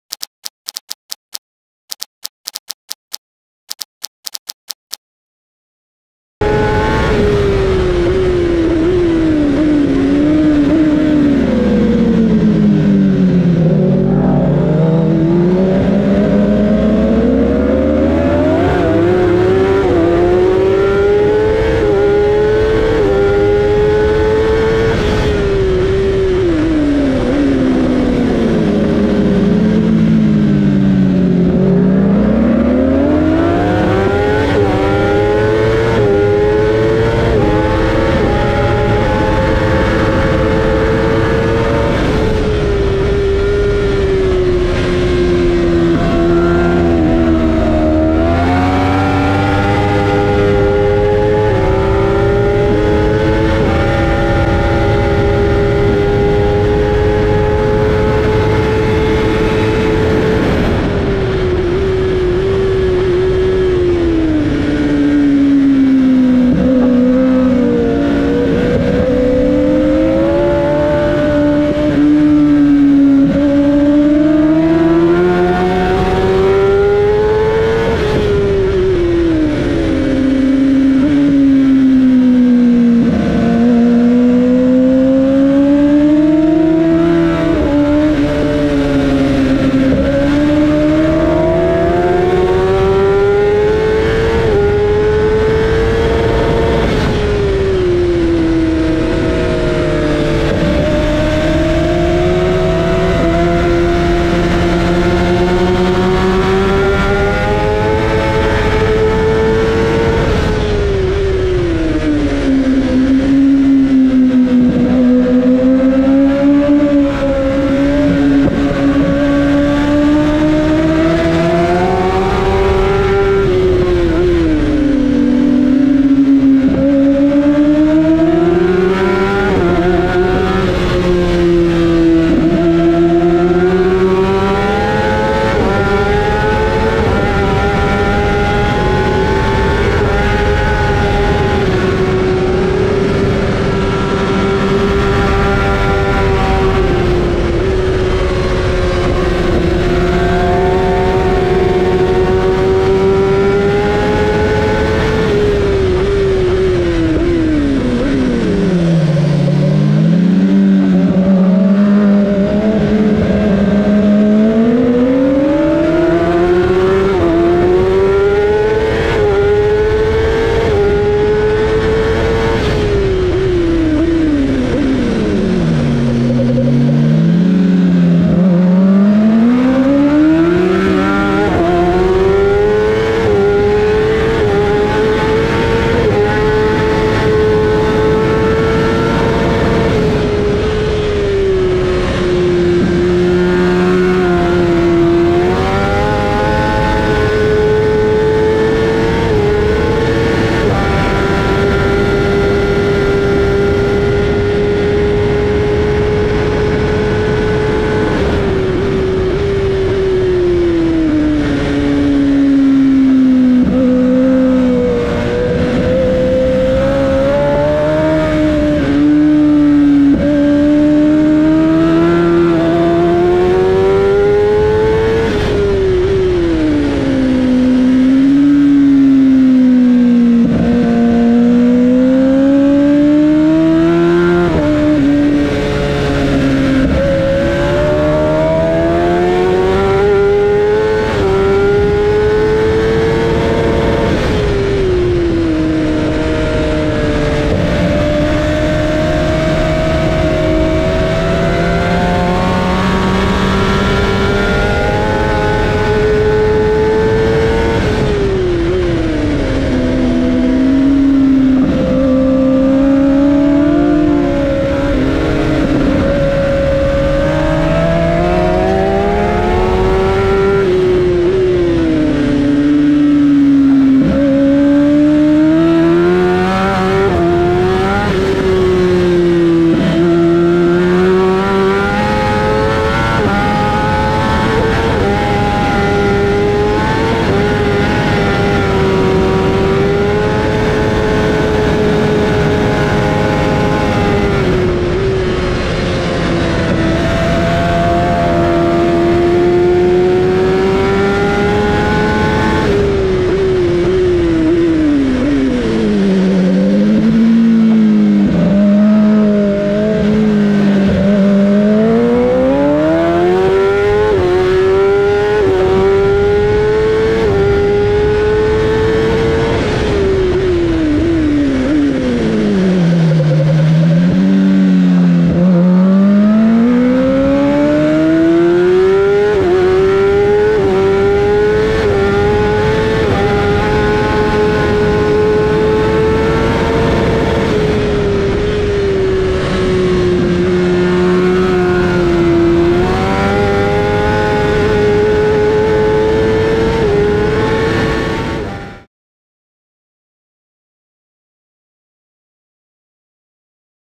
SPA - FRANCORCHAMPS ON BOARD 🤯 - KAWASAKI ZX-10R - GAMARINO
Dopo il primo giro a SPA sono rimasto SENZA PAROLE! Il perché lo potete vedere nel video...l'Eau Rouge, i curvoni a 300 km/h, le staccate violentissime e gli oltre 176 km/h di media rendono SPA unica!
Io e Kevin Calia strapazziamo le Kawasaki del Team 33 April Louit Moto nella leggendaria SPA - FRANCORCHAMPS!